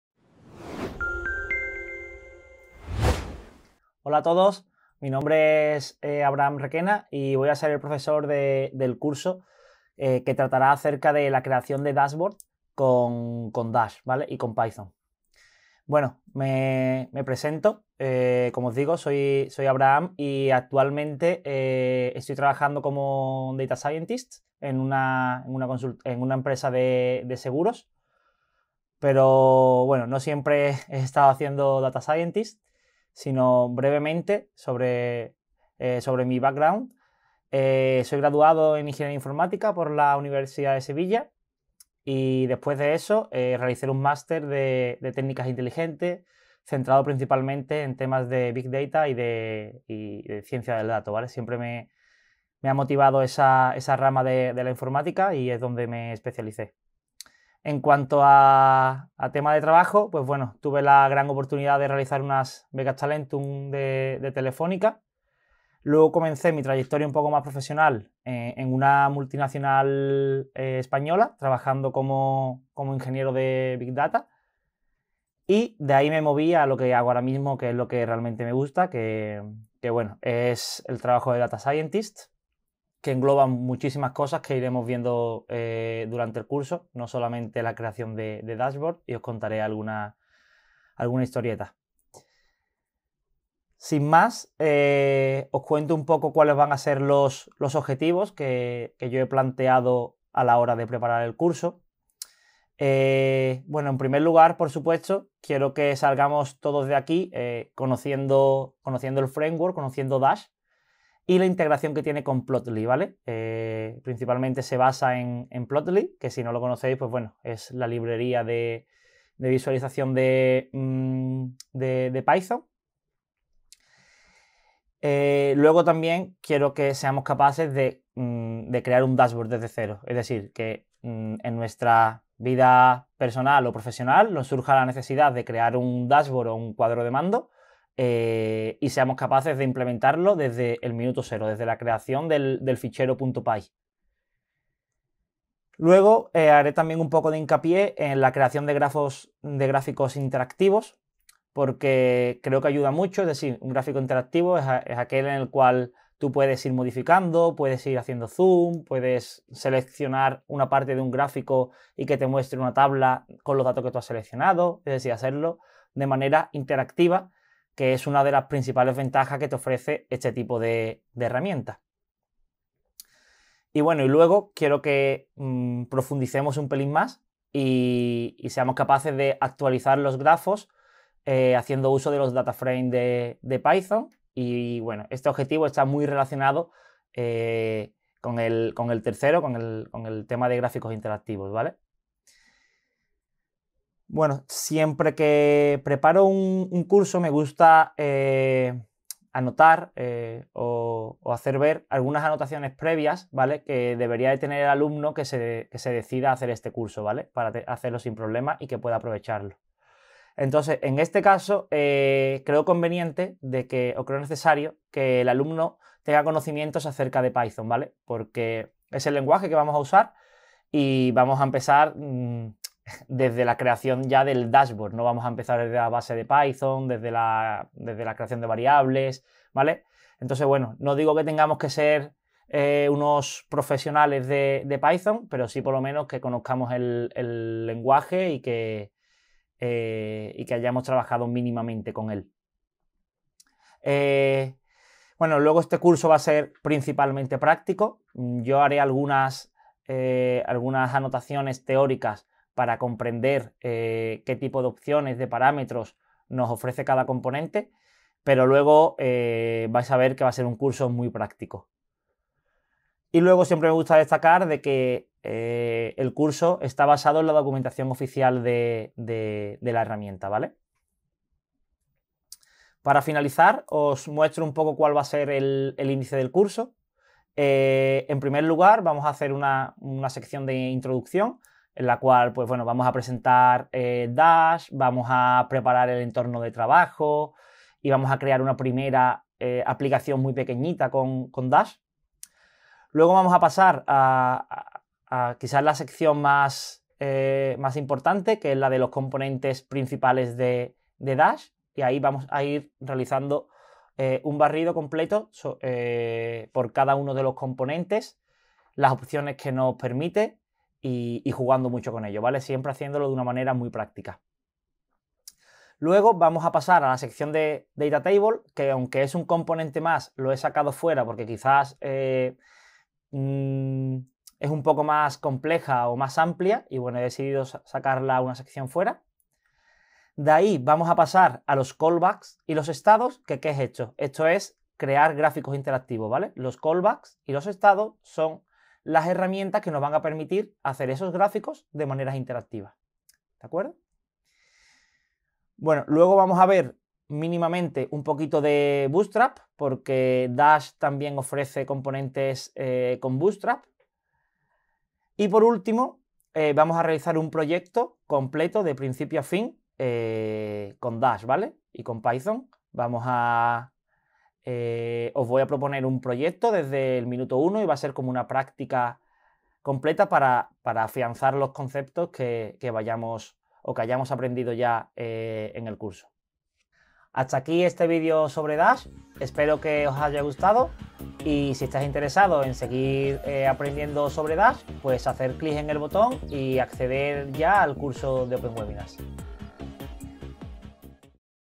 CURSO DE CREACIÓN DE DASHBOARDS CON DASH
En este Curso aprenderás cómo crear todo tipo de dashboards utilizando el framework Dash y el apoyo del lenguaje de programación Python.

- Crear dashboards desde cero.
- Conocer todos los layouts disponibles.
- Qué es plotly.
- Utilizar los callback’s para la actualización de los gráficos.
- Crear gráficos completamente interactivos.